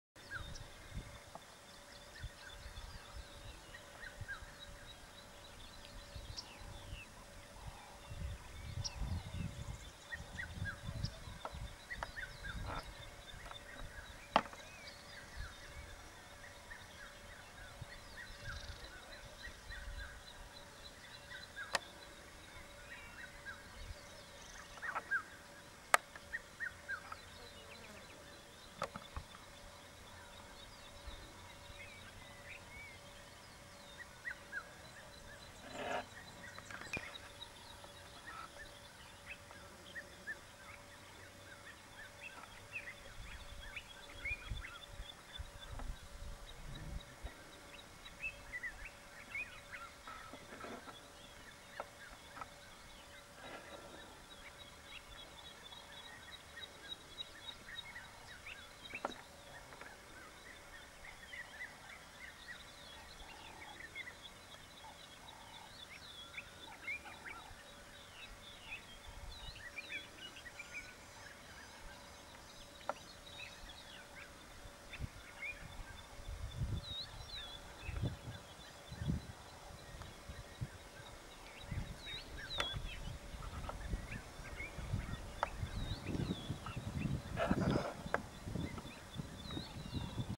Niespodziewane spotkanie z guźcami (Phacochoerus africanus).
Mpofini Game Lodge koło Vryheid (RPA) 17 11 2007.